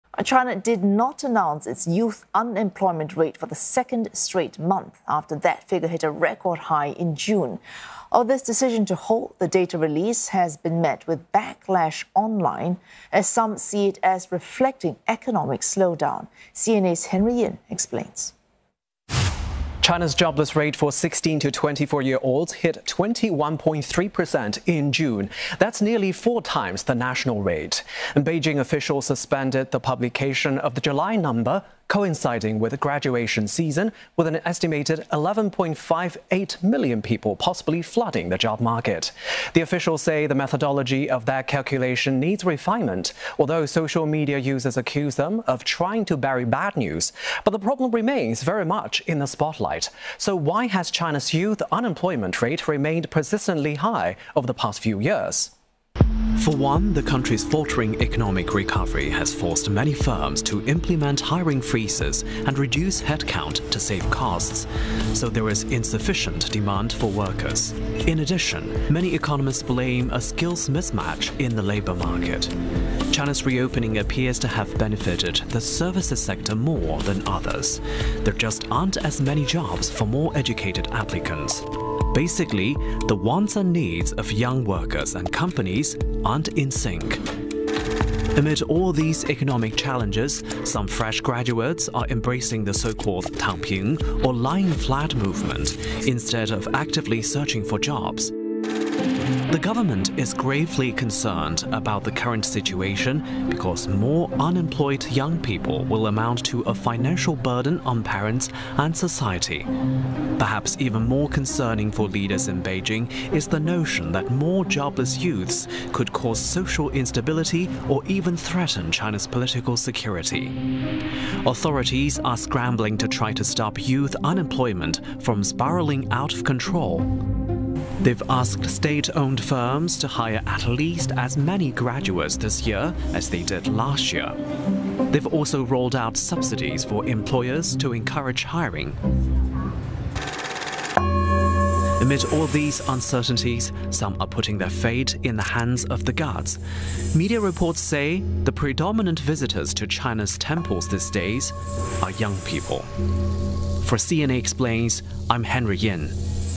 A look at China's youth unemployment rate
China did not announce its youth unemployment rate for the second straight month, after that figure hit a record high in June. The decision to halt the data release has been met with backlash online, as some see it as reflecting an economic slowdown.